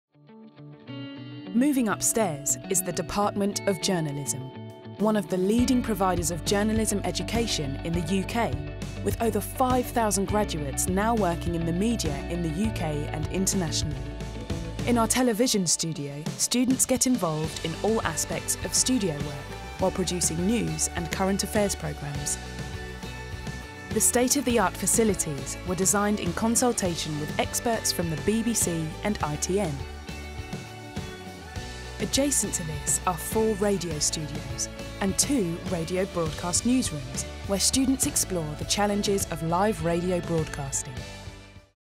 City, University of London Department of Journalism Tour
Explore some of our specialist Journalism facilities City, University of London. These facilities were developed in consultation with experts from the BBC and ITN, and were praised by the BJTC. Our facilities include: a television studio, four radio studios, two radio broadcast newsrooms, two digital newsrooms and two TV editing and production newsrooms